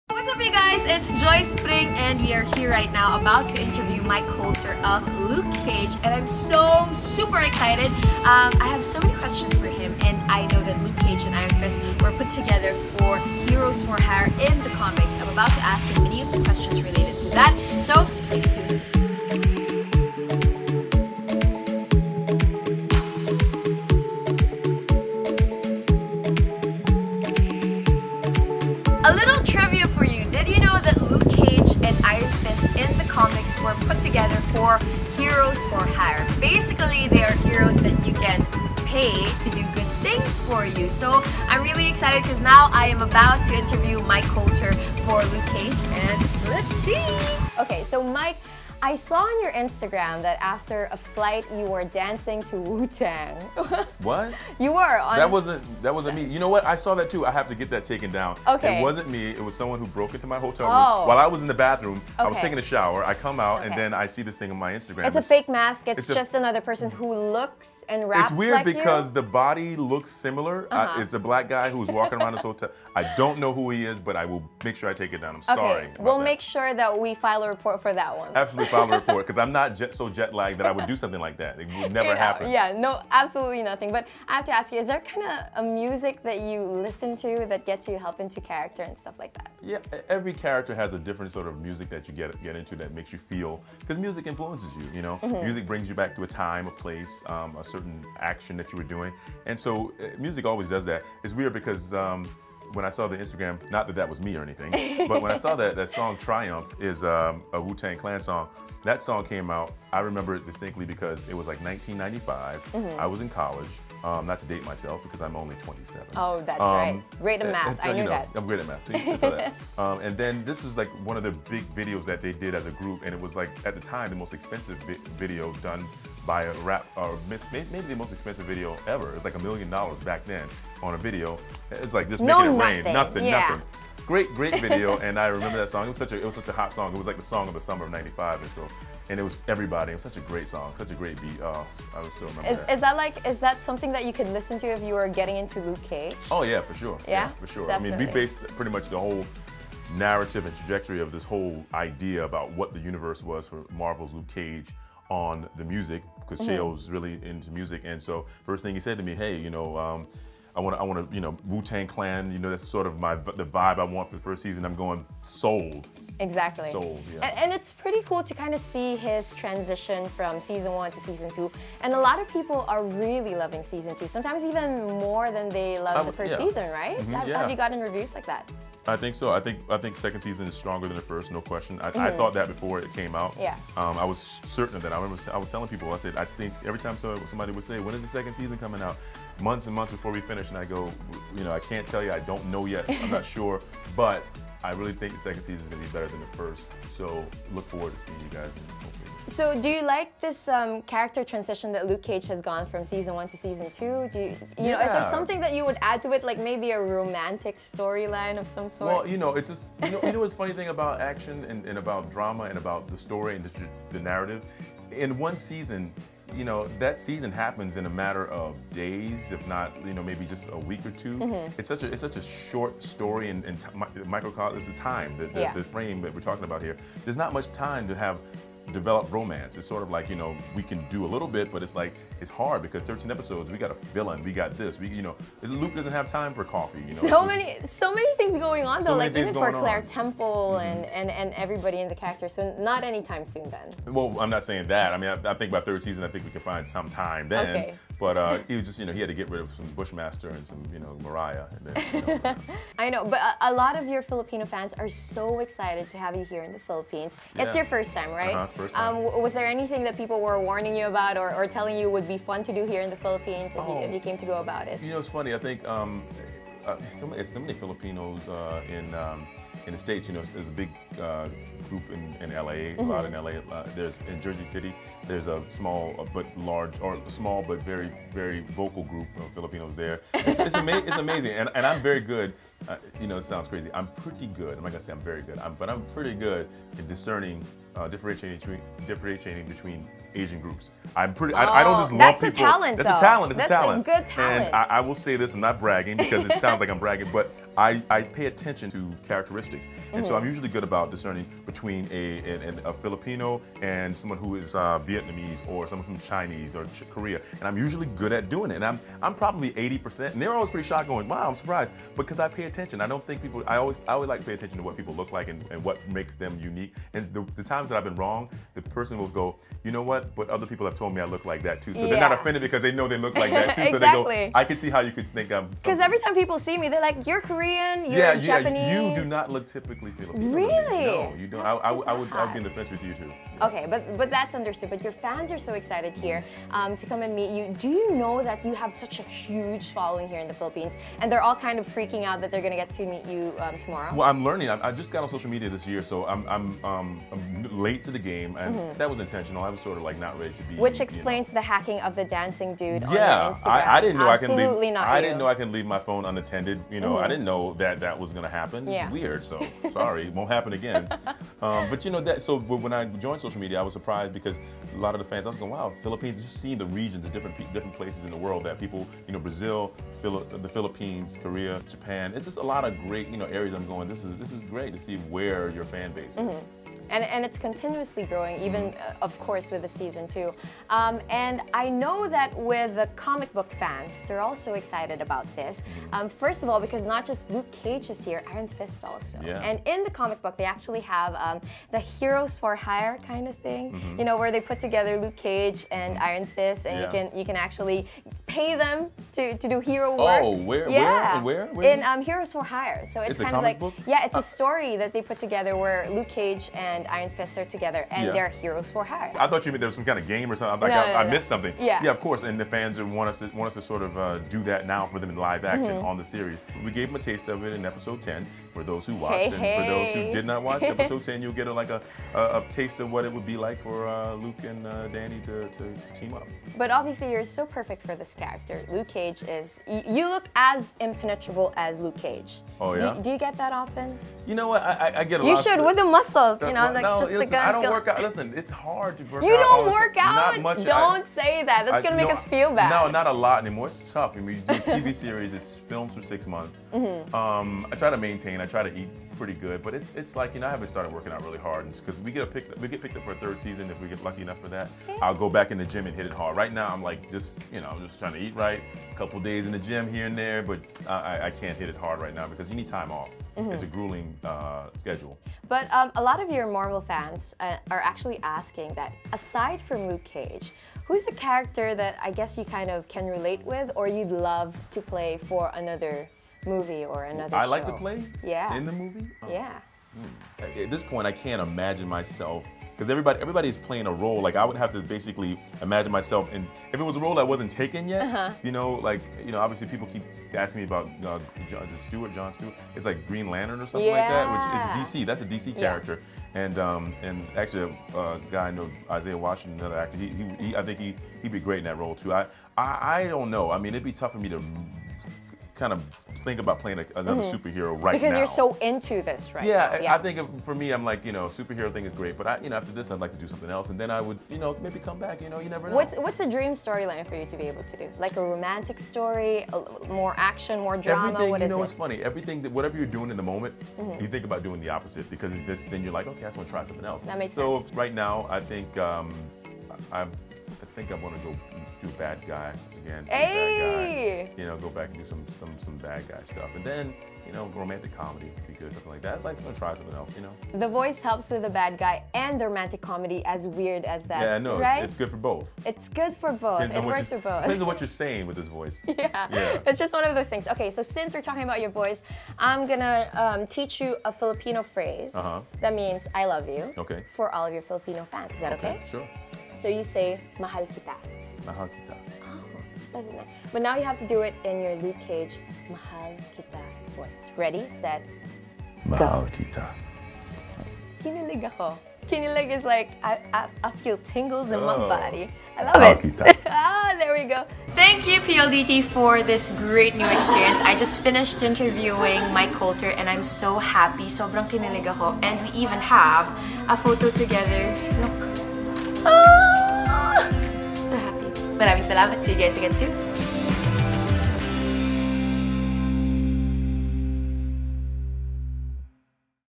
PLDT Home exclusive Netflix interview with Mike Colter of Luke Cage
Ever wonder how Mike Colter prepares for his iconic role in the Netflix series Marvel's Luke Cage? Or what he has to say to his Filipino fans?

Find out as our PLDT Home Netflix Insider Joyce Pring sits down for an exclusive interview with Mike Colter.